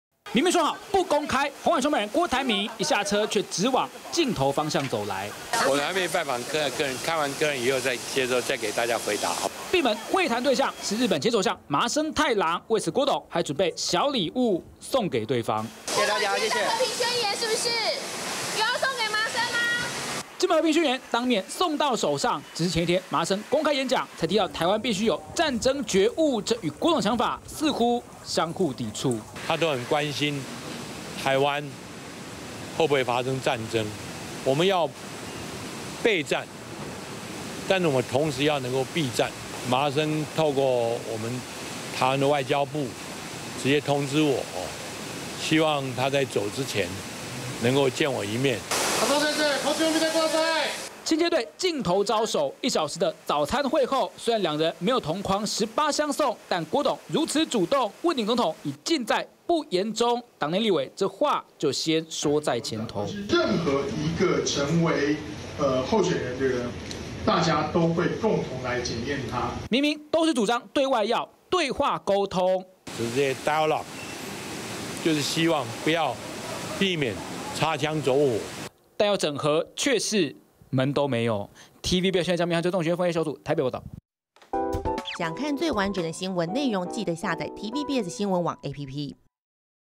麻生才籲「要有戰爭覺悟」 郭面贈「和平宣言」｜TVBS新聞
日本前首相麻生太郎，昨天在公開演講中，強調要避免戰爭，台灣要有「戰爭的覺悟」；不過，今天一早，他在台北與鴻海創辦人郭台銘碰面，郭台銘就帶上自己起草的「金門和平宣言」要送給麻生！